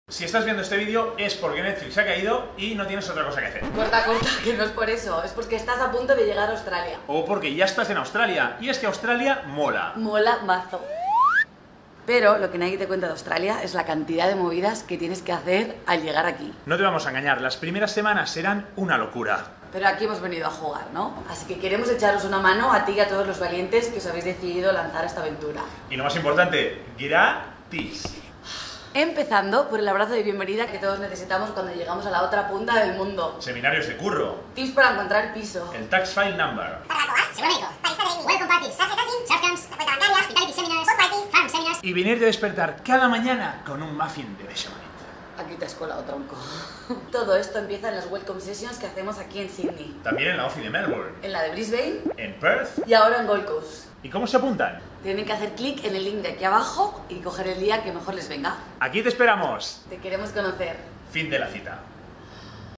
Si vienes a Australia con la Work and Holiday te ayudamos con todo!
Para todos aquellos que estáis a punto por fin de comenzar la experiencia de vuestra vida en Australia con la Work&Holiday visa o acabáis de aterrizar, Go Study también puede ayudaros! 👇

Si llegas a Sydney, Melbourne, Brisbane, Perth o Gold Coast, acércate a nuestras oficinas de Go Study y te ayudamos con todos los trámites de los primeros días (TFN, ABN, cuenta de banco, teléfono móvil, ABN) a buscar trabajo y a conocer gente (grupos de Whatsapp y a través de los eventos que vamos organizando).

¡¡Buen viaje y nos vemos pronto!!